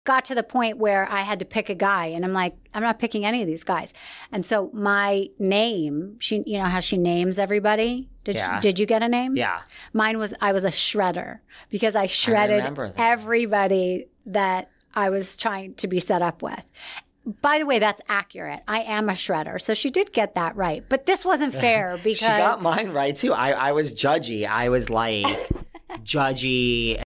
David & Rachel talk names Patti gave them millionairematchmaker @behindthevelvetrope
David Yontef and Rachel discuss the names that Patti gave them on Millionaire Matchmaker

Ep. 8